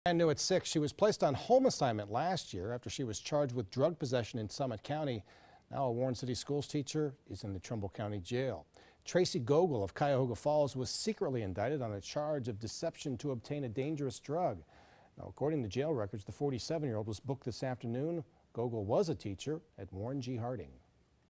Former Warren teacher jailed on drug charges
The Warren City Schools teacher was placed on home assignment last year after she was charged with drug possession in Summit County.